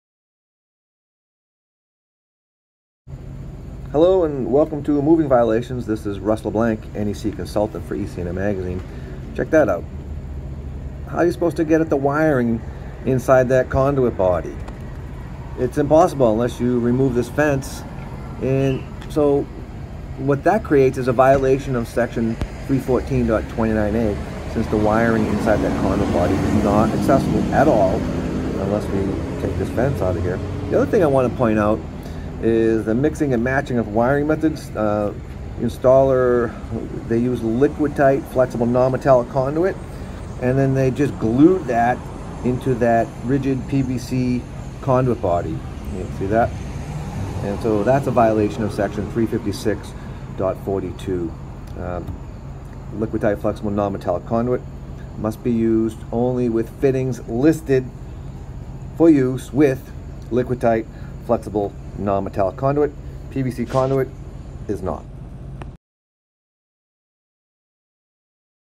Moving Violations Video No. 295: Mission Impossible
In this episode, Russ examines some wiring inside of a conduit body that is impossible to reach. This installation is a violation of Sec. 314.29(A) which requires boxes and conduit bodies to be installed so that the wiring can be accessed without removing any part of the building or structure. In addition to that violation, the installer used some mixing and matching of wiring methods which caused a violation of Sec. 356.42.